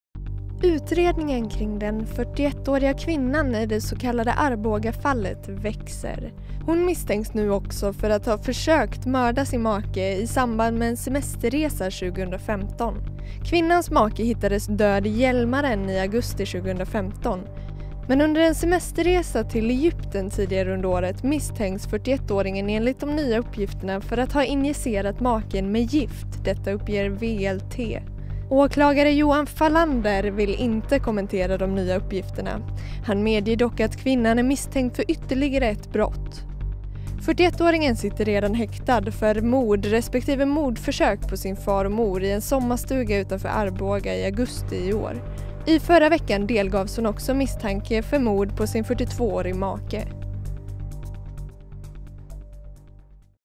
Nya misstanken mot Arbogakvinnan
Utredningen kring den 41-åriga kvinnan i det så kallade Arbogafallet växer. Hon misstänks nu också för att ha försökt mörda sin make vid en semesterresa.